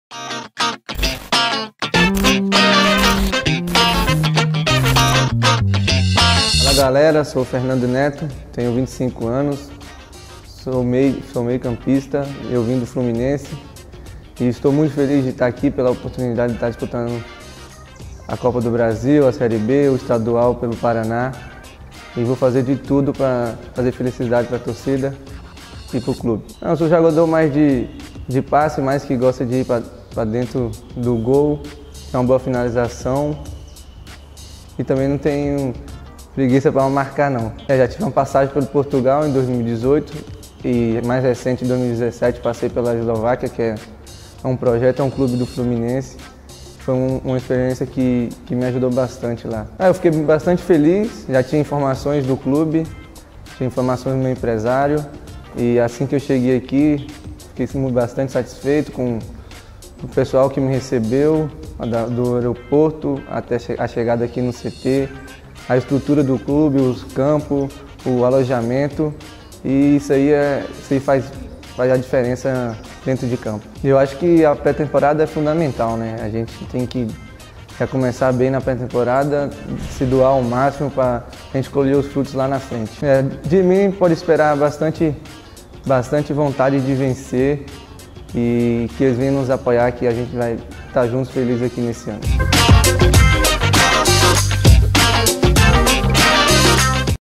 SEJA BEM-VINDO, FERNANDO NETO
Fernando Neto é mais um meio-campista que reforça nosso elenco. Fala aí, Fernando.